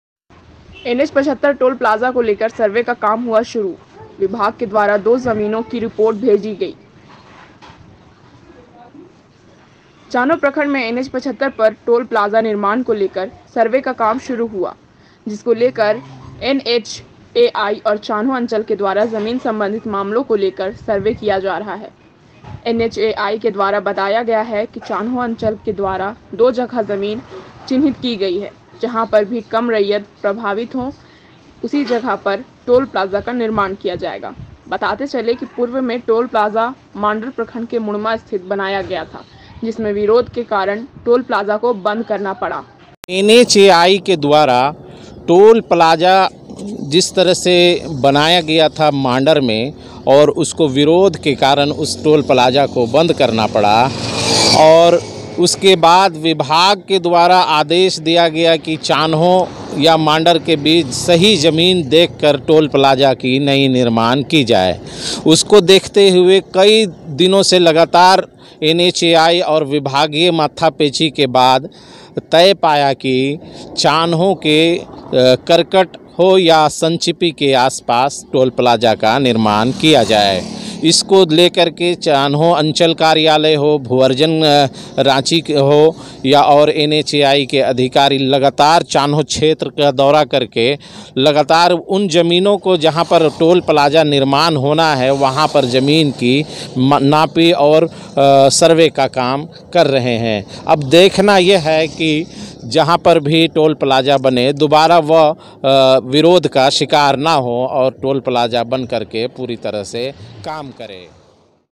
NH75 टोल प्लाजा को लेकर सर्वे का काम हुआ शुरू विभाग के द्वारा दो जमीनों की रिपोर्ट भेजी गई है
चान्हो_प्रखंड में एनएच_75 पर टोल_प्लाजा_निर्माण को लेकर सर्वे का काम शुरू हुआ जिसको लेकर एनएचएआई और चान्हो अंचल के द्वारा जमीन संबंधित मामलों को लेकर सर्वे किया जा रहा है एनएचएआई के द्वारा बताया गया है कि चान्हो अंचल के द्वारा दो जगह जमीन चिन्हित की गई है जहां पर भी कम रैयत प्रभावित हों है उसी जगह पर टोल प्लाजा का निर्माण किया जाएगा बताते चलें कि पूर्व में टोल प्लाजा मांडर प्रखंड के मुड़मा स्थित बनाया गया था जिसमें विरोध के कारण टोल प्लाजा को बंद करना पड़ा।